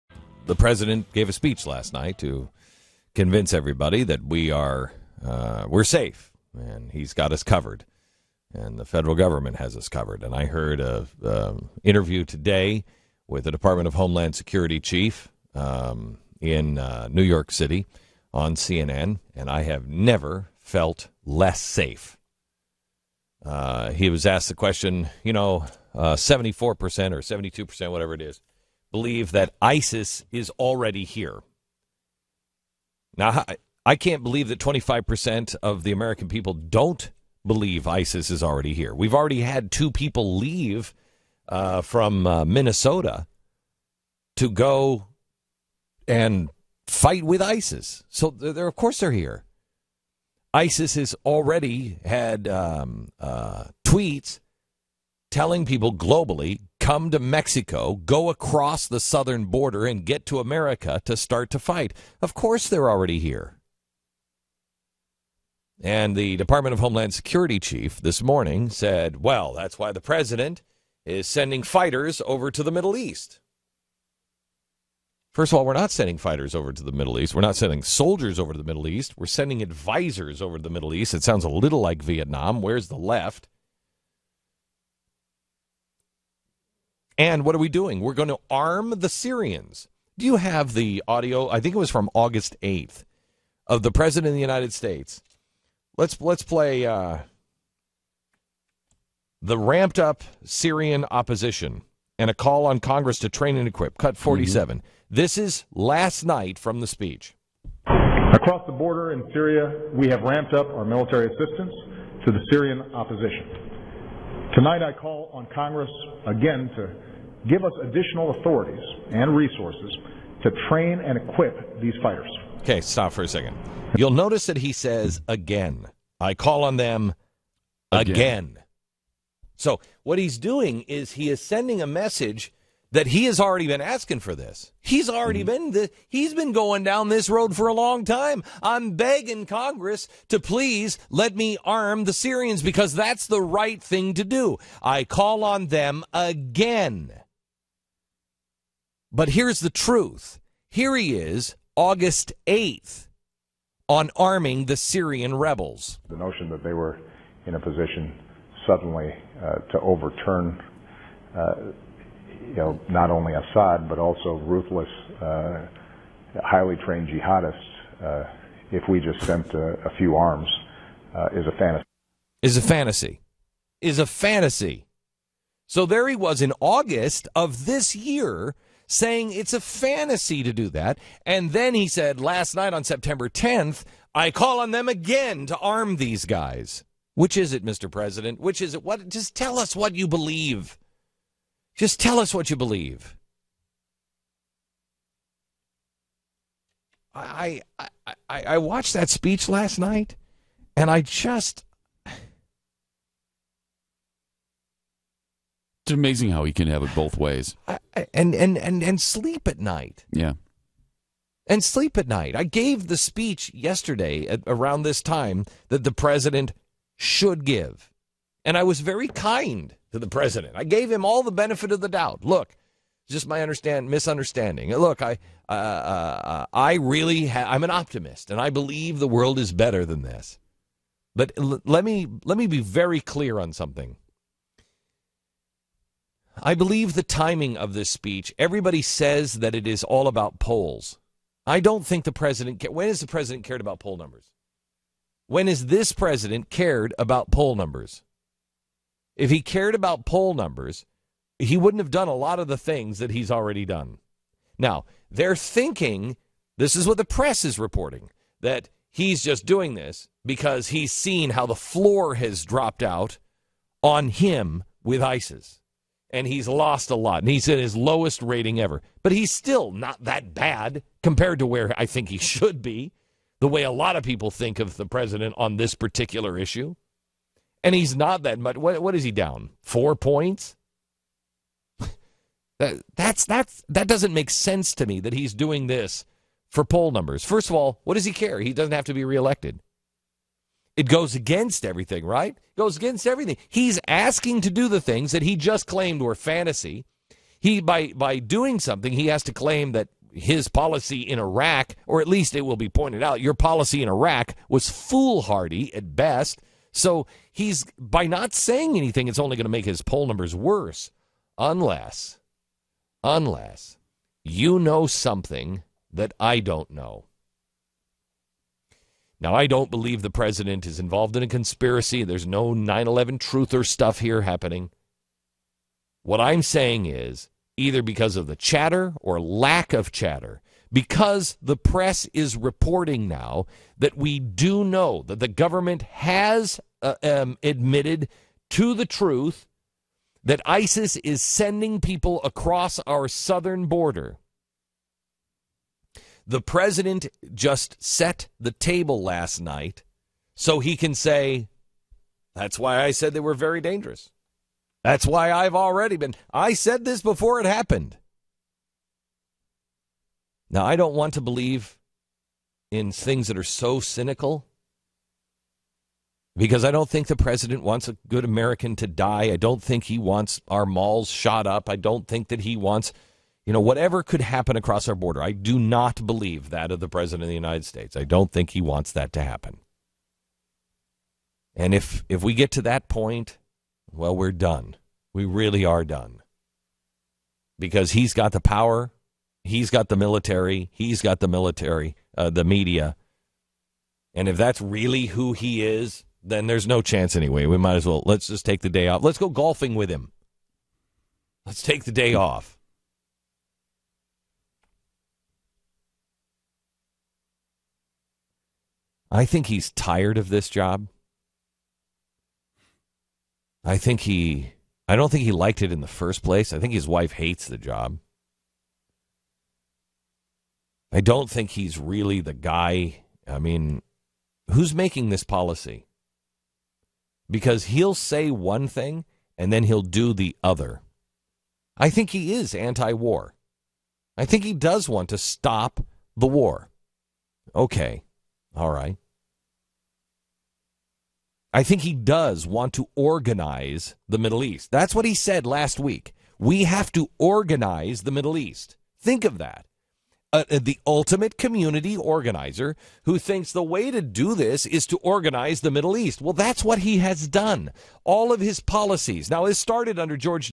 Glenn Beck: Reaction To Obama Speech
Glenn reacts to President Obama's speech about his plan to deal with ISIS.